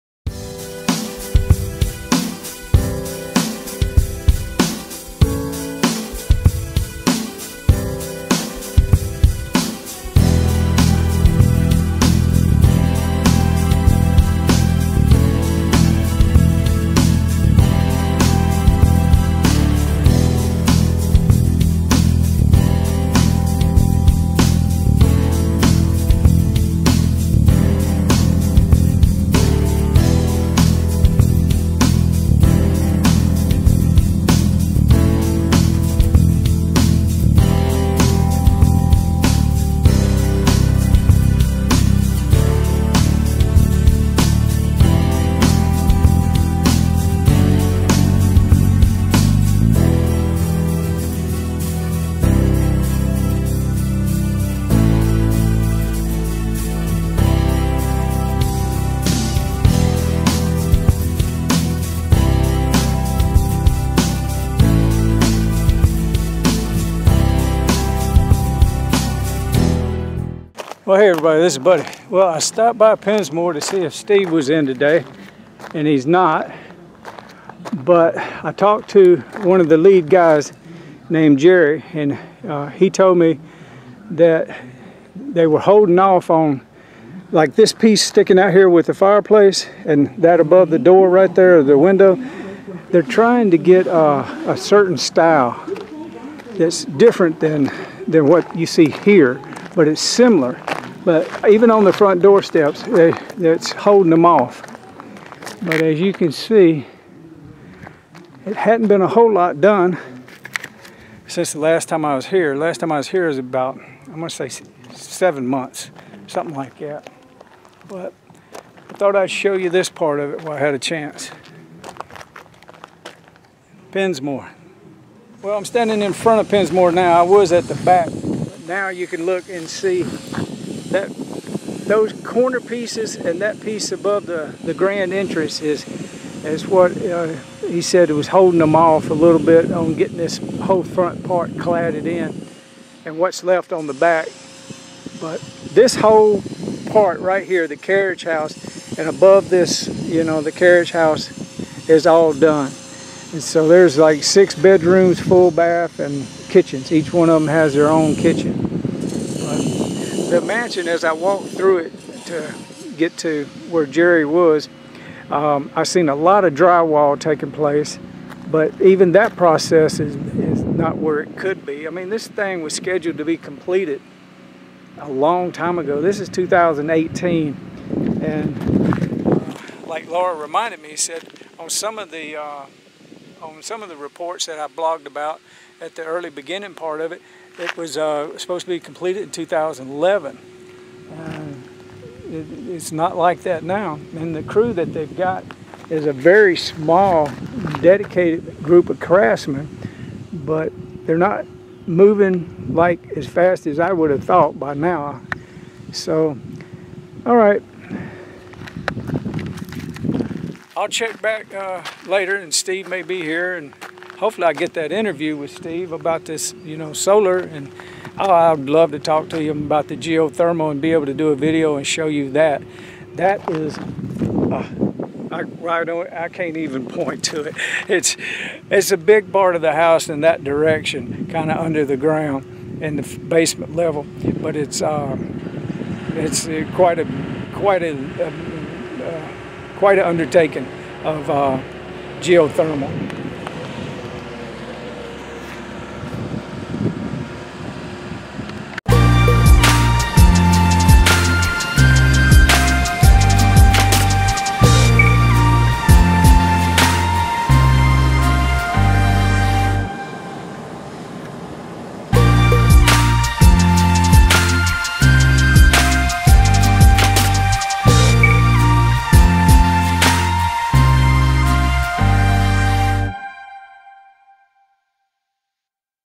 Chateau Pensmore, Could this be the new White House? Update May 11th 2018
Update on the Pensmore Mansion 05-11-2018
Pensmore, The 72,000-square-foot Ultimate Tornado-Proof Home under construction in Highlandville, Missouri

This is the latest video I could find to share with you. It’s unfinished and still MINDBLOWING!

In the Midwest, Steve Huff is aiming to build the ultimate tornado-proof home. Mr. Huff, a software entrepreneur, is building a 70,000-square-foot reinforced-concrete, chateau-style home in rural Christian County, Mo. The home, known as Pensmore, has 12-inch-thick walls and ballistic-proof windows that have been tested to withstand the equivalent of a two-by-four board traveling at 40 miles an hour, mimicking the speed at which debris can be hurtled during a big storm. "If a tornado came up, you'd be foolish to leave this house," Mr. Huff says.
Mr. Huff, who is an investor in a concrete company, says that the home will also be resistant to intruders: "It would be like trying to drill your way through a bank vault."
When it is completed in early 2014, it will be one of the largest private homes in America. Mr. Huff says he plans to have lots of extended family stay with him, as well as visiting writers, scientists and other students who may want to study the safety and environmentally sustainable technologies used to construct the home.

Chateau Pensmore: Settlement Reached In $63M

~ Buddha   : )